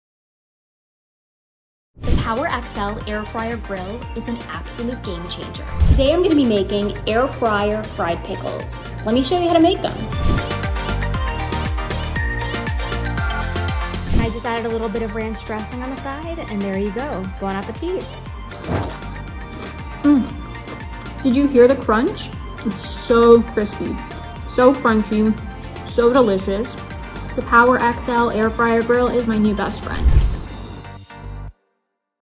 PowerXL Air Fryer Grill Review By Stephanie | Game Changing Air Fryer
Watch as Stephanie, a real PowerlXL Air Fryer Grill user, explains how she thinks this air fryer is a game-changer. She takes only 10 minutes to cook these air-fried pickle recipes to make them crispy, crunchy, and delicious! This air fryer can be anyone's new best friend!